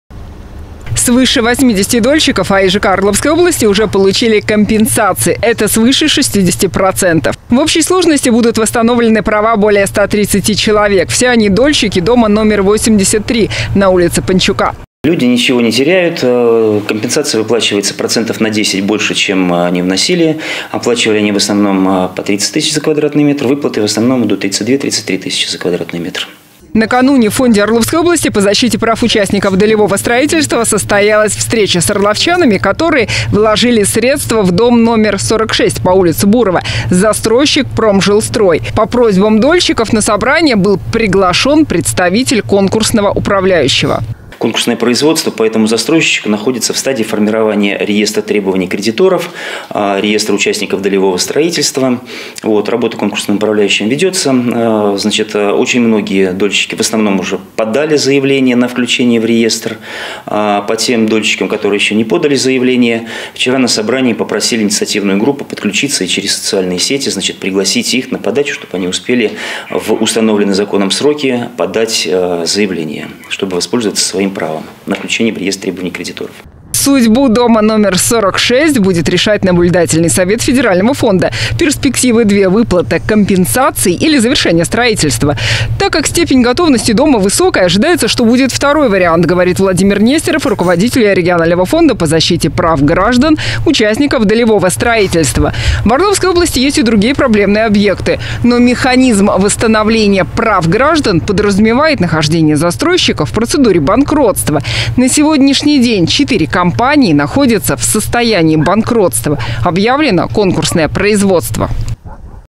В Орле решается судьба недостроенного дома
"Фонд Орловской области по защите прав граждан-участников долевого строительства" провёл встречу с "обманутыми дольщиками", купившими квартиры в доме по Бурова, 46. Перспективы - либо достраивать здание, либо выплачивать компенсации. Первый вариант более вероятен, так как дом находится в высокой степени готовности.